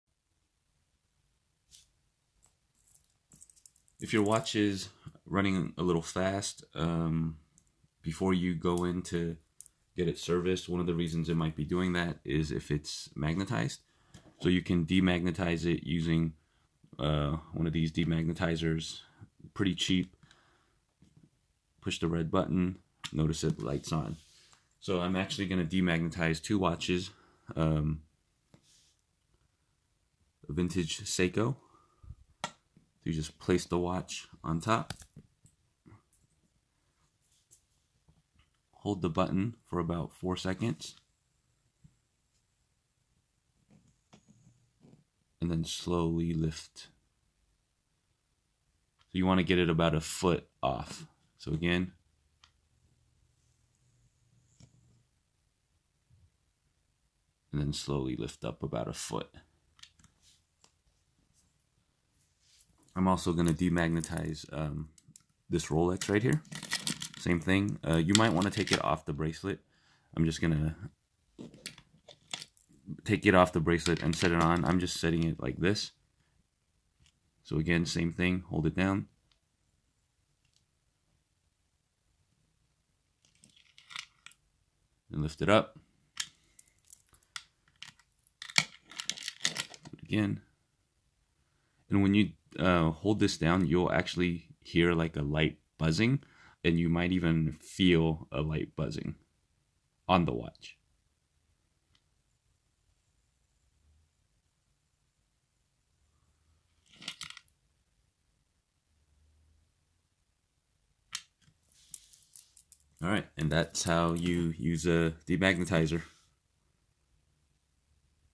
HD How to use Demagnetizer Seiko Rolex Automatic Mechanical watch Demagnetize Omega
Every time you grab a cold beer or do the laundry, you’re putting you precious wristwatch at risk. Many everyday electronics and appliances — your speakers, your refrigerator, your computer, your microwave, etc. — produce magnetic fields. When subjected to these fields, the metal parts inside your watch movement become magnetized, throwing off the accuracy of your timepiece.